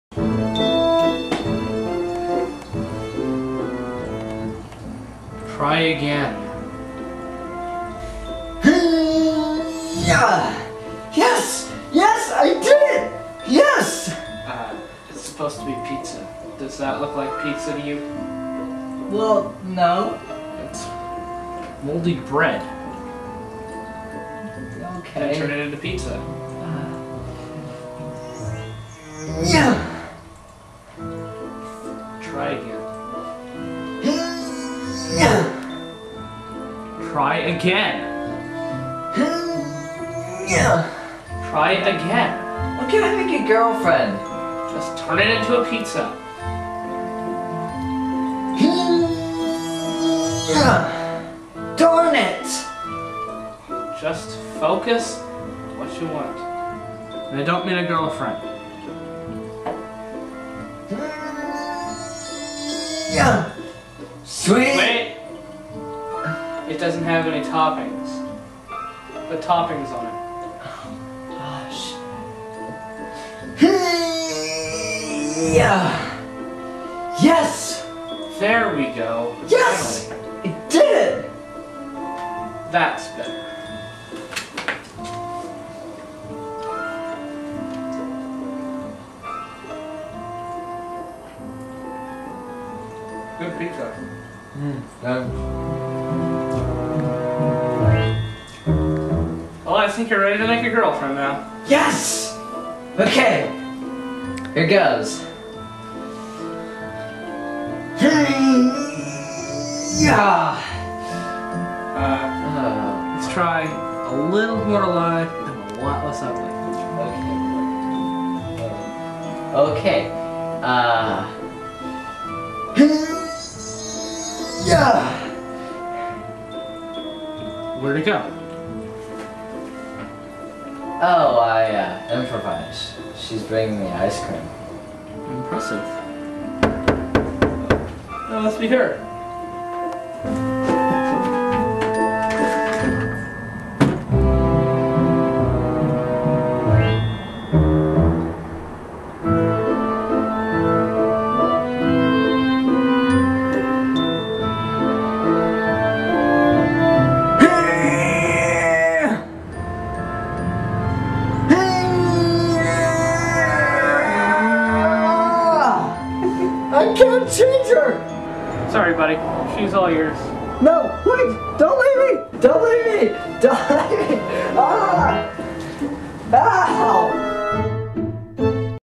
Superpowers 2
This is the 2012 sequel to the 2011 original "Superpowers" which you can In this sequel, the younger brother tries to master the superpowers discovered by his older brother in the first video.

This video was submitted into the 2012 Golden Bulb Awards at Cornerstone Church.